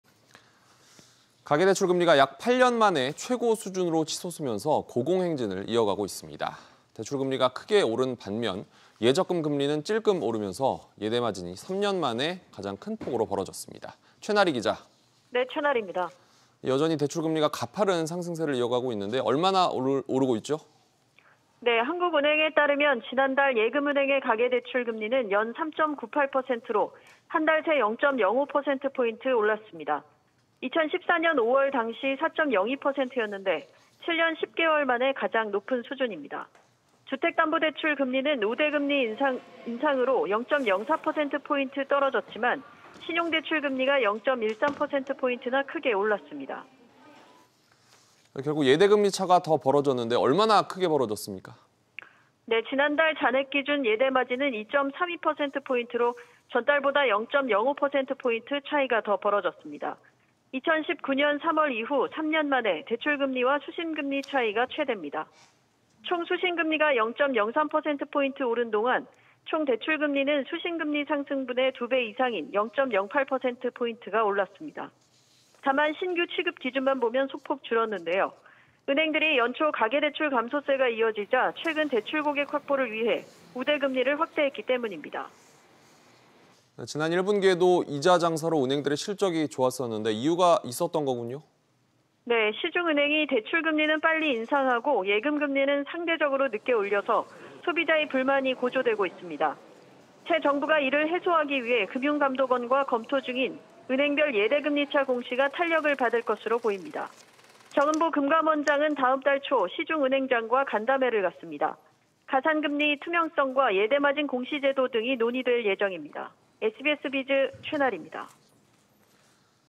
가계대출금리 7년10개월 만에 최고…‘예대마진’ 3년 만에 ‘최대’
[앵커]

가계대출 금리가 약 8년 만에 최고 수준으로 치솟으며 고공행진을 이어가고 있습니다.

대출금리가 크게 오른 반면 예적금 금리는 찔끔 오르면서 예대마진이 3년 만에 최대폭으로 벌어졌습니다.

최나리 기자, 여전히 대출금리가 가파른 상승세를 이어가고 있군요?

[기자]

한국은행에 따르면 지난달 예금은행의 가계대출 금리는 연 3.98%로 한 달 새 0.05%p 올랐습니다.